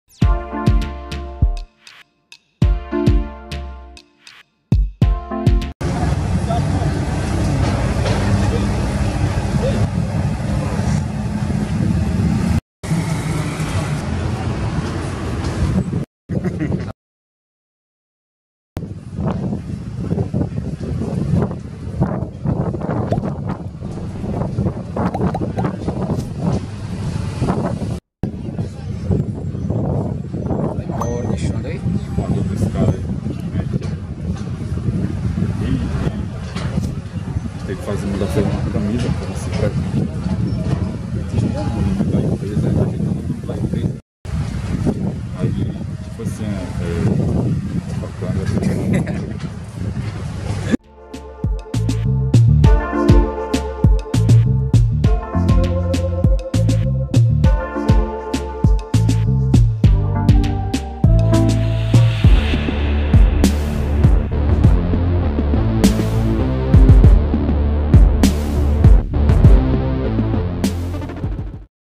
🔴 Saída do A.Nunes II do Porto do Rodwey
Saída do Navio A. Nunes II do Porto da Manaus Moderna